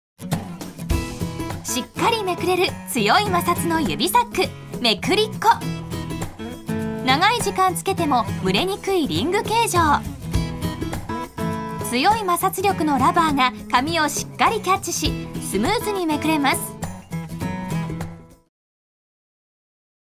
【PLUS】リング型 指サック メクリッコ
しっかりめくれる強い摩擦力のラバータイプ。ロングライフデザイン賞を受賞した、リング型指サック。